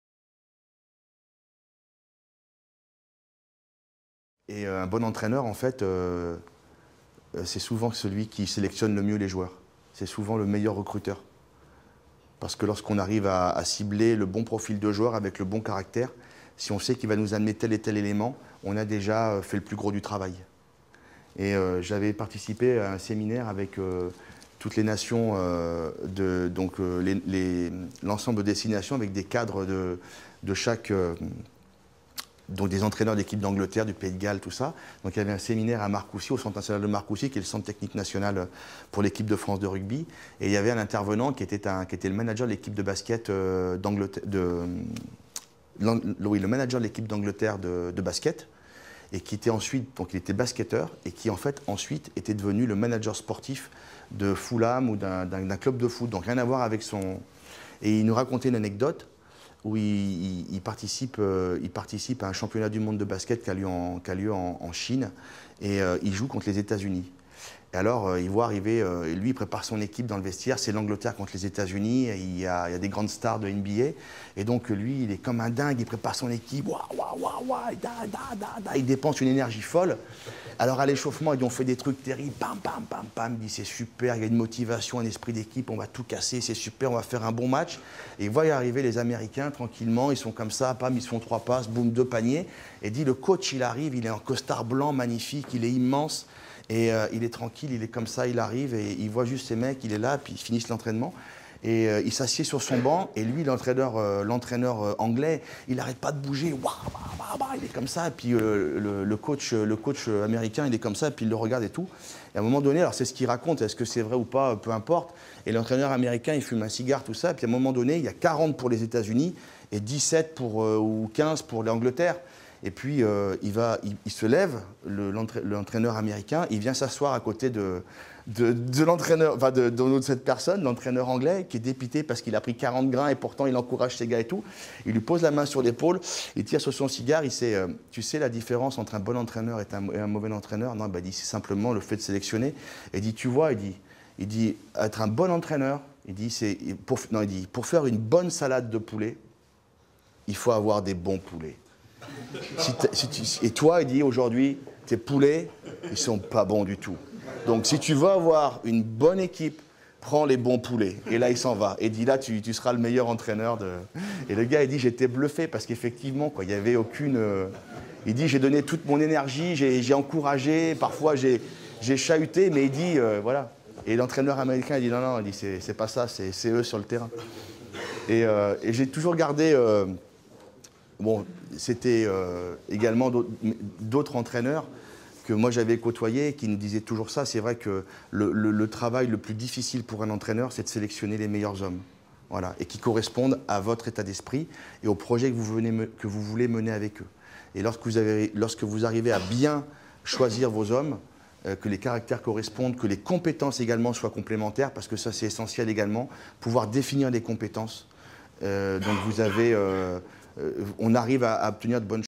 Fabrice Landreau : " Un bon entraîneur est un bon sélectionneur"
Anecdote de l'entraineur de Basket de l'équipe d'Angleterre sur la sélection de ses joueurs